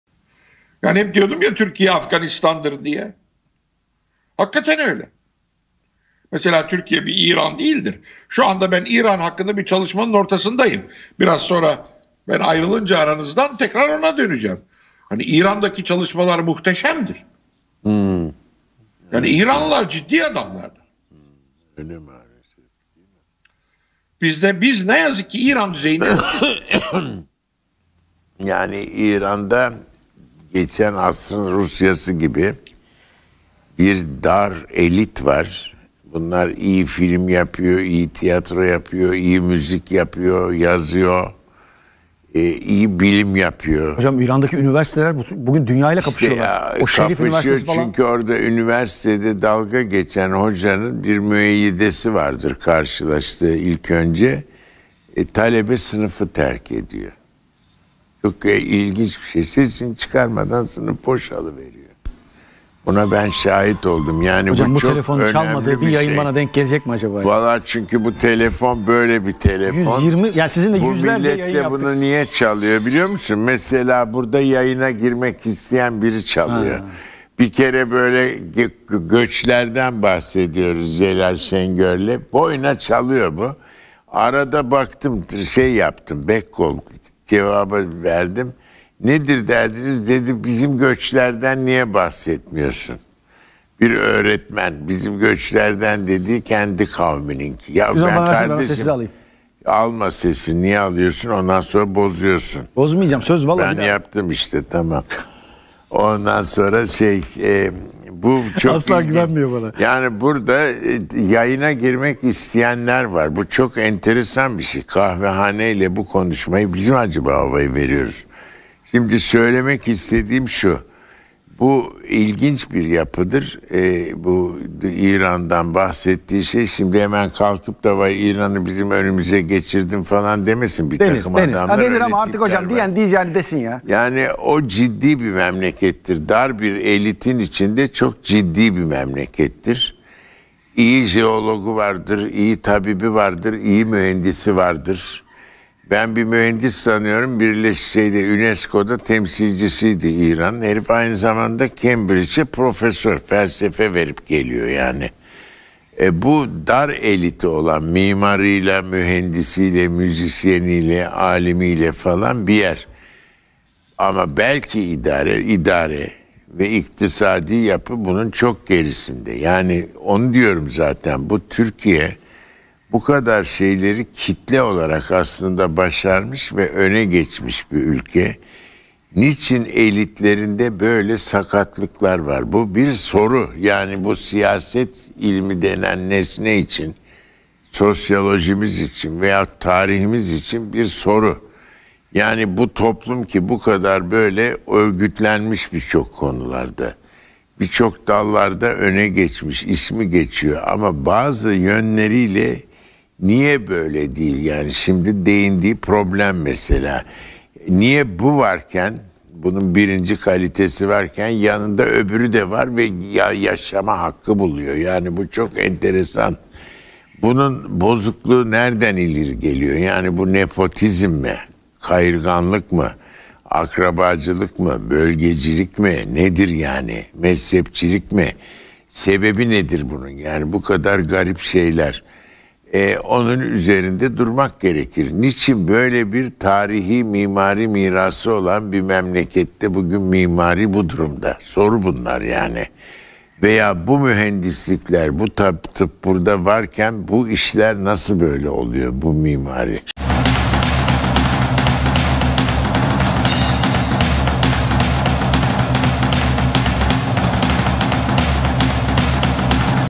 Mimari Neden Bu Durumda? Prof. Dr. İlber Ortaylı | Candaş Tolga Işık ile Az Önce Konuştum
Prof. Dr. Celal Şengör ve Prof. Dr. İlber Ortaylı'dan İran ve Türkiye karşılaştırması. Türkiye'de mimari neden bu durumda? Bu bozukluk nereden geliyor? | Candaş Tolga Işık ile Az Önce Konuştum